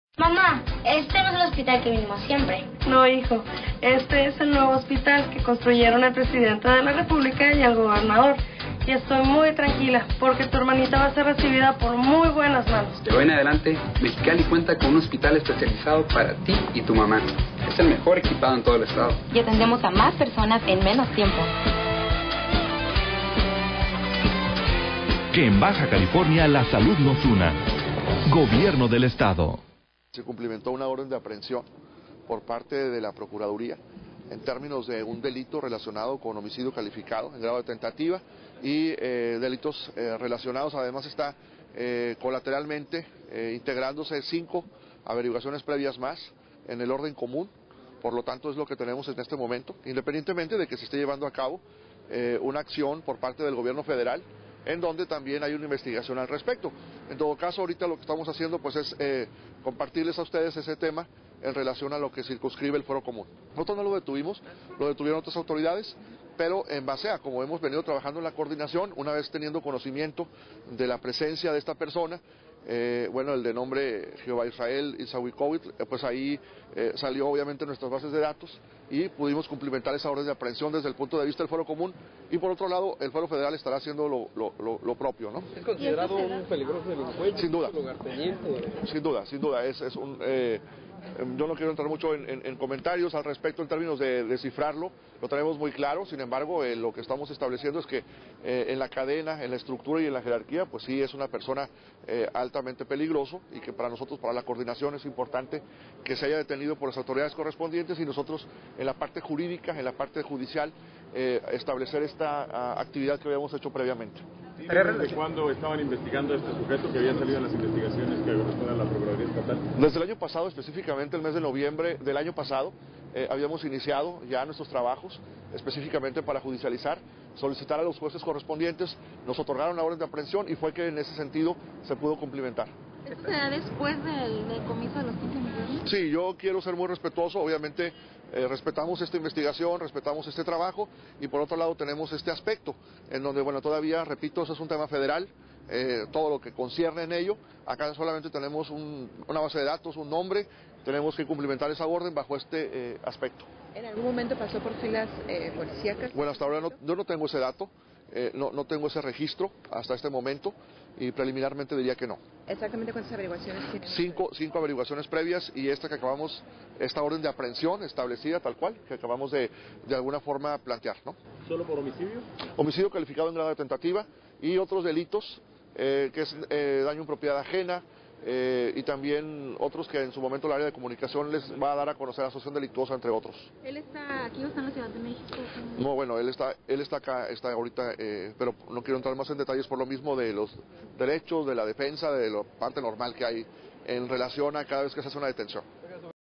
5 AVERIGUACIONES TIENE EL CUERVO EN LA PGJE DICE PROCURADOR ROMEL
Jehová  Israel Ilhuicatzi, apodado el Cuervo, fue capturado por las autoridades
del grupo de coordinación y se le cumplimentó una orden de aprehensión por
el delito de homicidio en grado de tentativa.